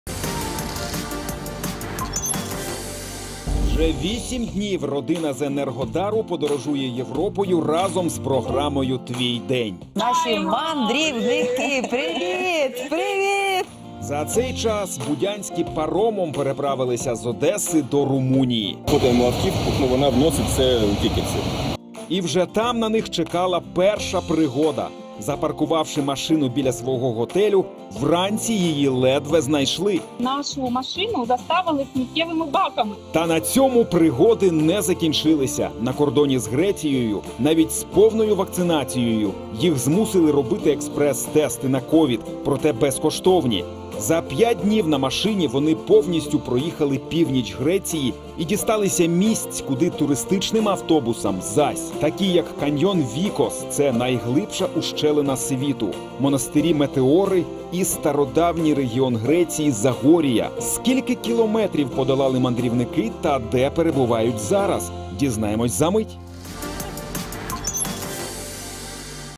Які пригоди пережила родина Будянських за перші 8 днів автоподорожі Європою
8 днів тому Твій день вирушив у подорож Європою. А запросили нас наші глядачі, родина  Будянських: Дмитро, Наталя та Карина. Вони за 38 днів планують проїхати 7 тисяч кілометрів. Які пригоди їм вже довелося пройти - дивіться в сюжеті.